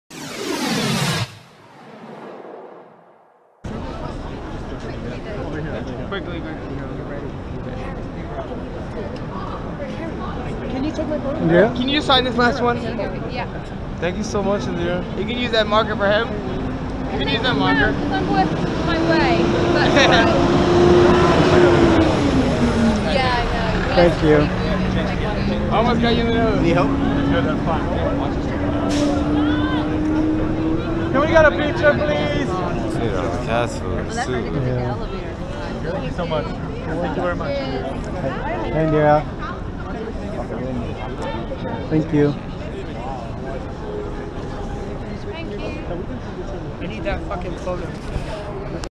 Indira Varma signs autographs for TopPix
in New York City.

For authentic autographed memorabilia of Indira Varma and many other celebrities, check out TopPix Autographs